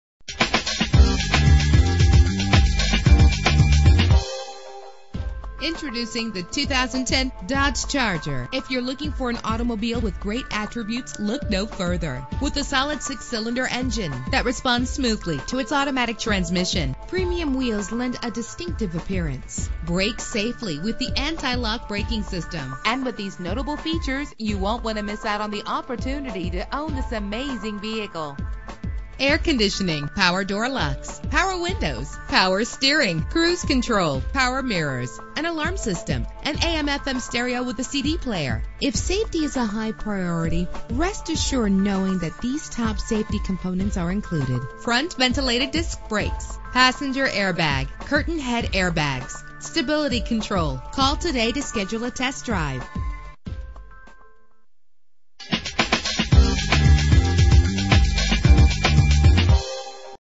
2010 Dodge Charger - Downersgrove IL
Year: 2010
Make: Dodge
Model: Charger
Trim: SXT
Engine: 3.5 liter 6 cylinder 24 valve
Transmission: 4-Speed Automatic
Color: White
Mileage: 26080
Address:
2311 West Ogden